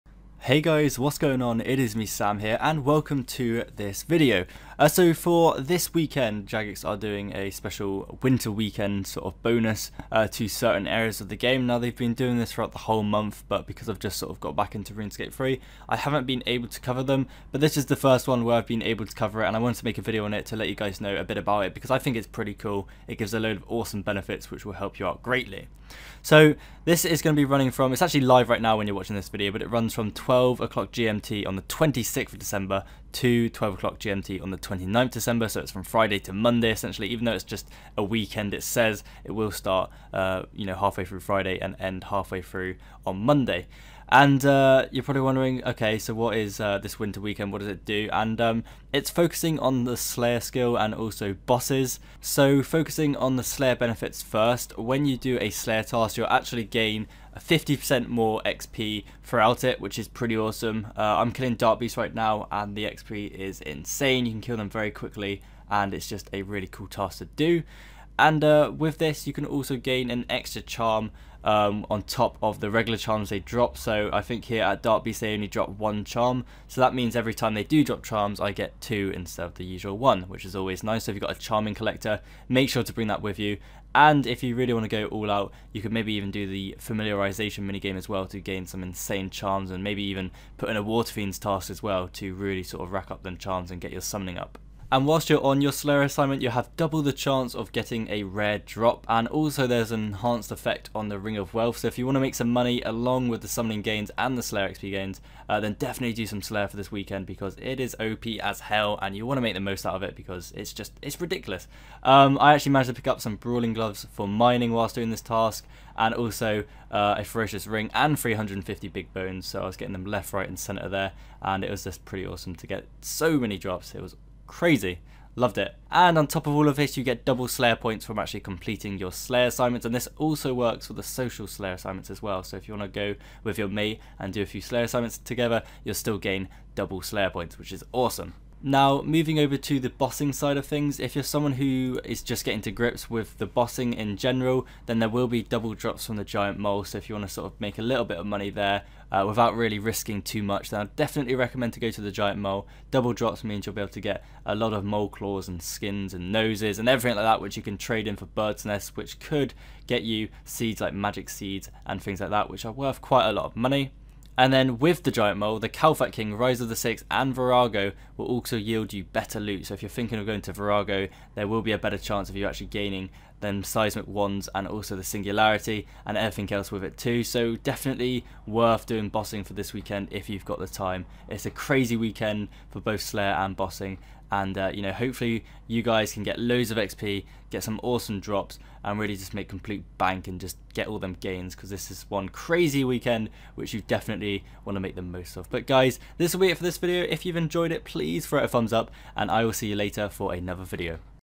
Slayer & Bossing Benefits - Winter Weekend
For this weekend we see the Slayer skill get some awesome benefits such as 50% more XP and increased rare drops. Whilst the PvM'ers get a a fair share of the spoils too with double loot from the Giant Mole and also a higher chance of better loot from Kalphite King, Vorago, Giant Mole and Rise of the Six.

These benefits will be running from 12:00 GMT on 26th December to 12:00 GMT on 29th December.